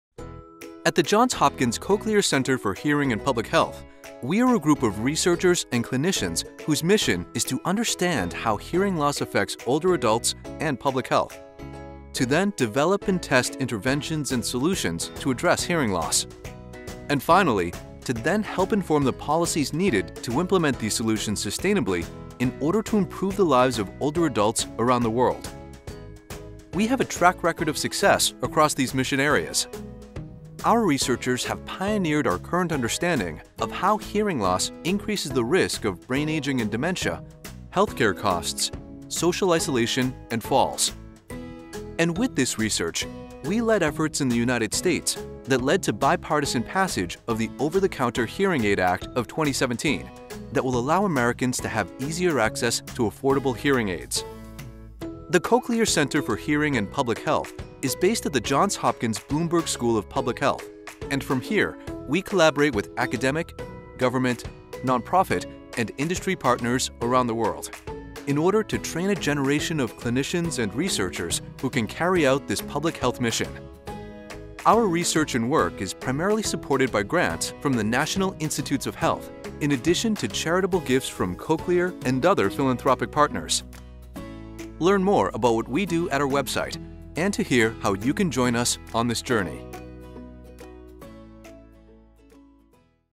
2. What We’re Doing at the Cochlear Center
Learn about what we are doing at the Johns Hopkins Cochlear Center for Hearing and Public Health!